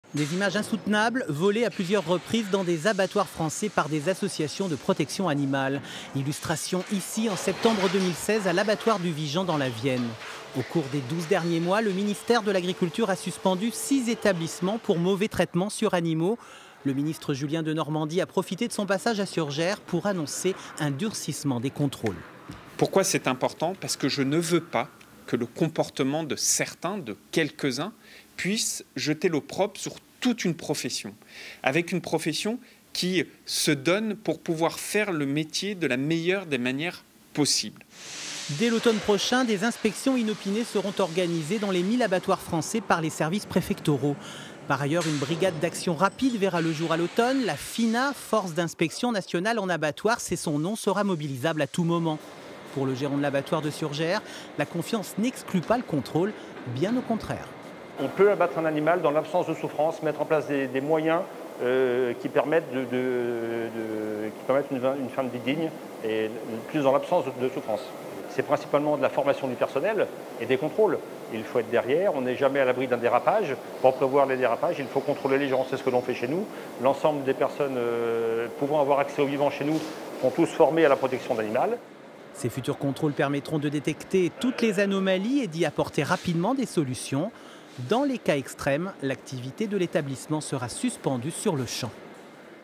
Surgères : renforcement des contrôles dans les abattoirs
Julien Denormandie, le ministre de l'Agriculture, était en visite à Surgères ce lundi 5 juillet pour annoncer 115 millions d'euros d'investissement et un renforcement des contrôles dans les abattoirs. Une brigade spéciale va être installée pour veiller au bien être animal.
Reportage Frédéric Cartaud, Cédric Cottaz et Martine Sitaud
Intervenants :
- Julien Denormandie,  ministre de l'Agriculture et de l'Alimentation
- Nicolas Joyet, gérant société Surgérienne d'Abattage